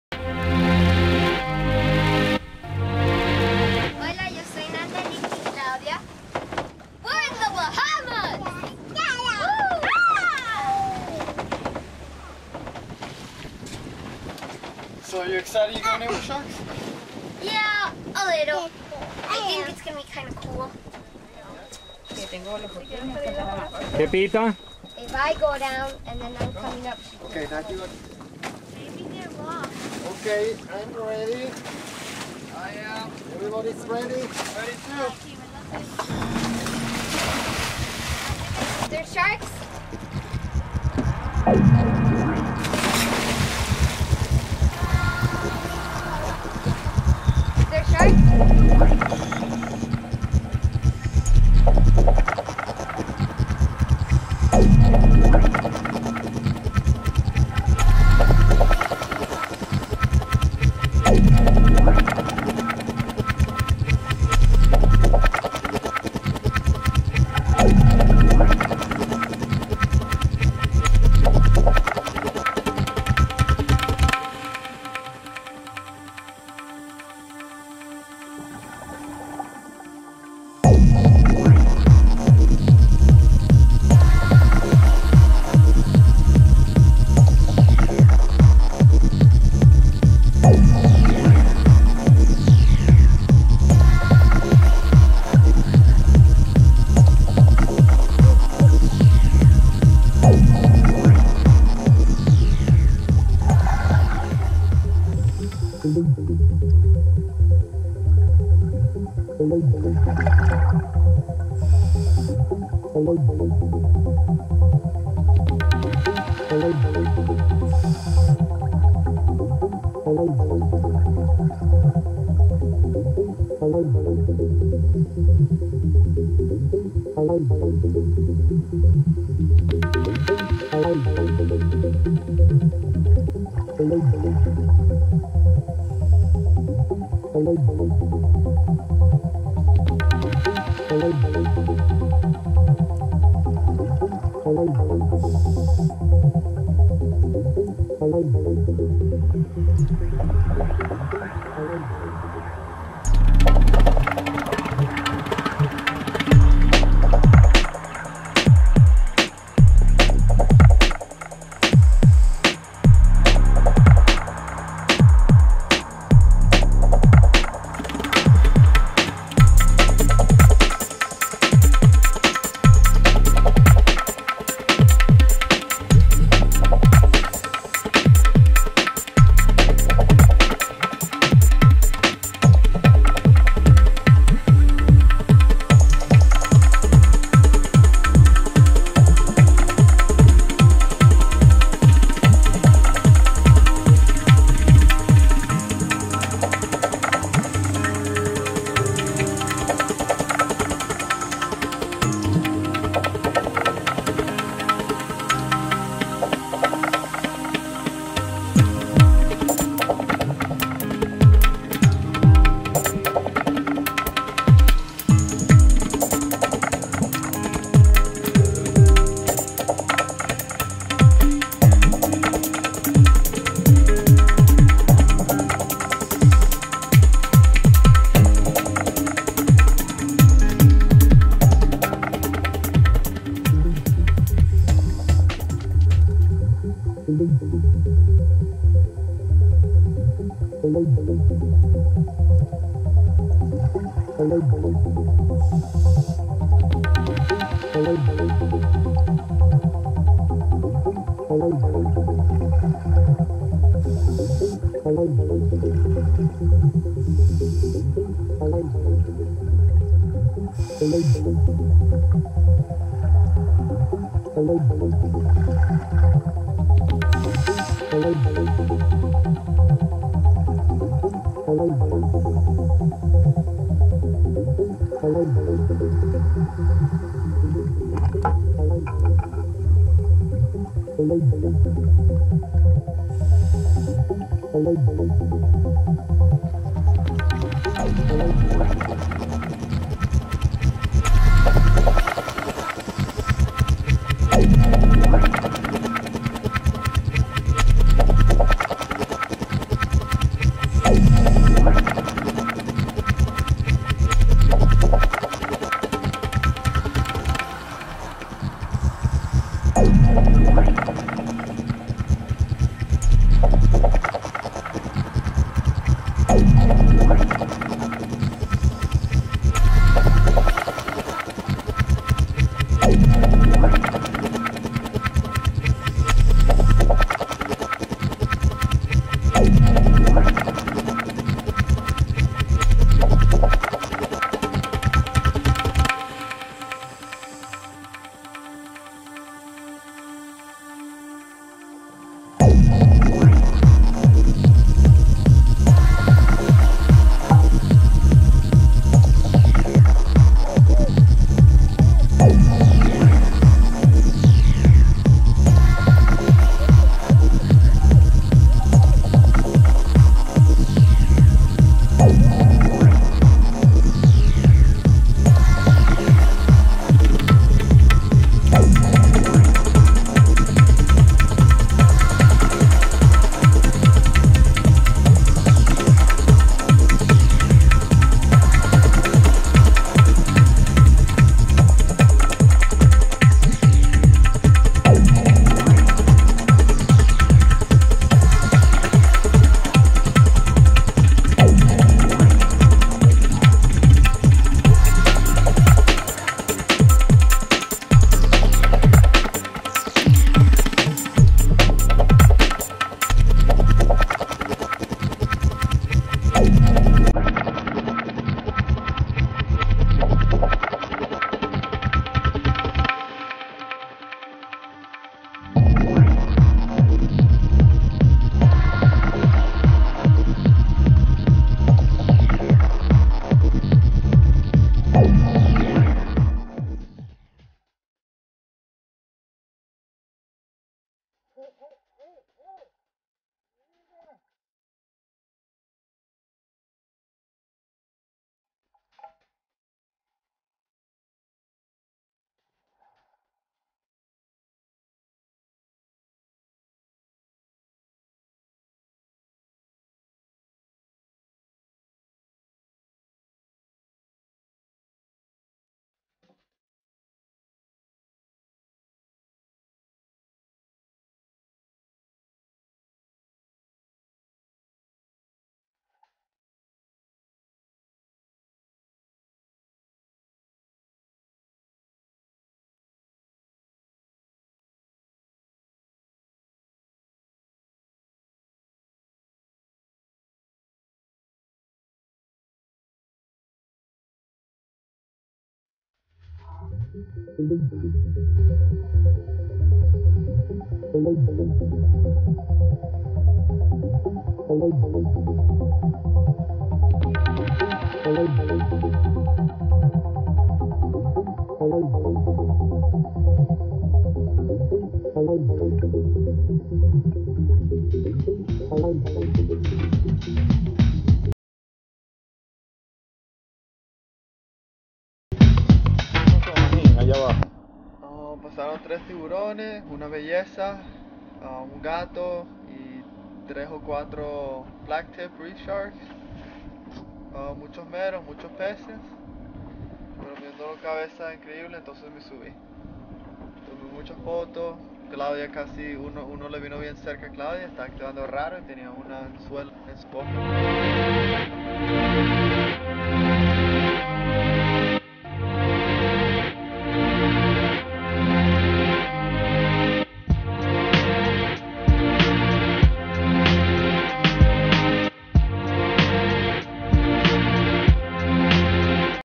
Teenage Girls Diving with Sharks in Underwater Family Adventures The Bahamas.
The Llaneza family kids Magin 19, Claudia 13, Nathalie 12 years old in the video, August 2007.
Have a lot of fun and enjoy diving with sharks in the Bahamas Islands. A lot of fun!
We need to support the worldwide effort to give sharks a chance to survive. Diving with sharks has been a unique experience. Hopefully some of you will try it and experience the adventure just to be in close encounter with yourself and with this unique & amazing animal. We all need to learn more and understand why they attack and end the fear that drives us to destroy them. Perhaps most important of all is that the sharks are in serious trouble. More than 100 million of them are killed by people every year through greed, need, selfishness, ignorance, and a depletion of marine ecosystems that sharks are defenseless against. The threatened extinction of many shark species is no longer fiction but grim reality. Their disappearance will inevitably lead to an ecological disaster in the seas and a tragedy for humans.